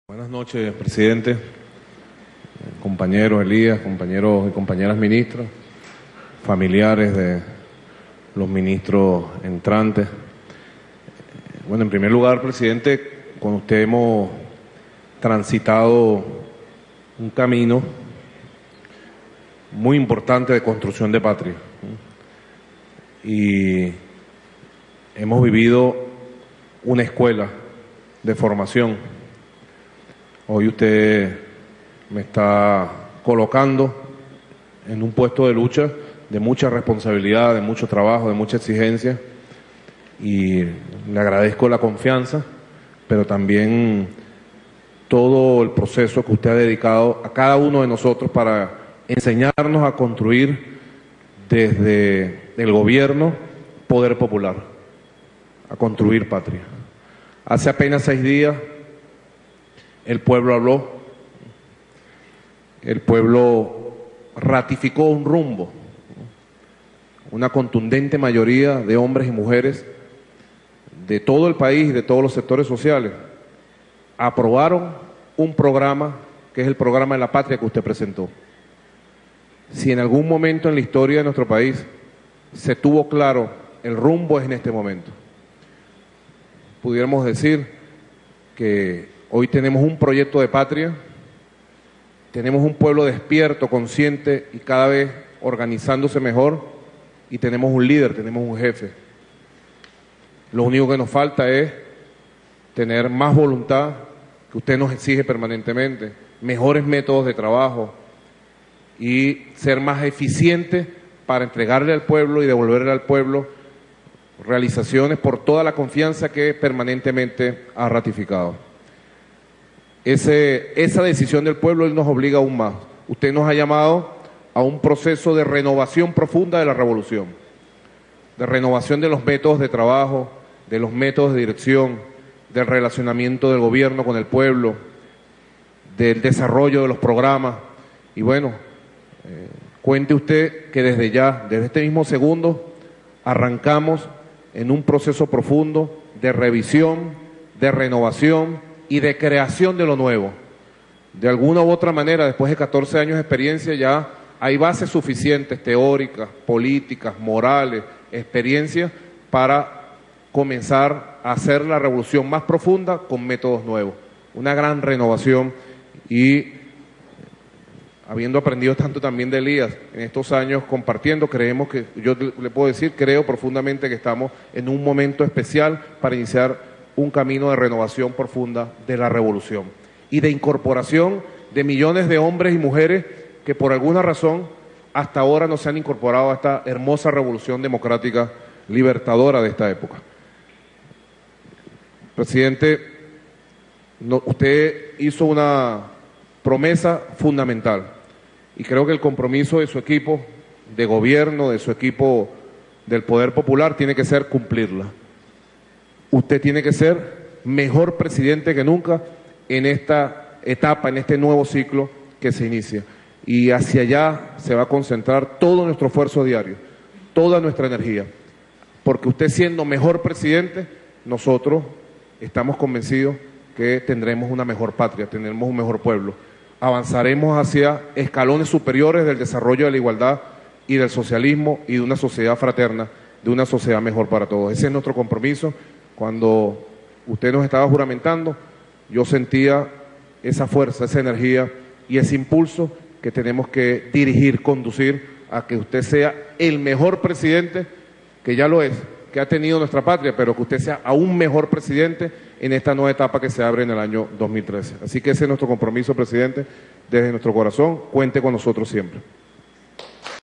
Nicolás Maduro fue juramentado como nuevo Vicepresidente de la República.
Desde el Palacio de Miraflores, el jefe de Estado, Hugo Chávez, preside el acto de juramentación de Nicolás Maduro como nuevo Vicepresidente de la República.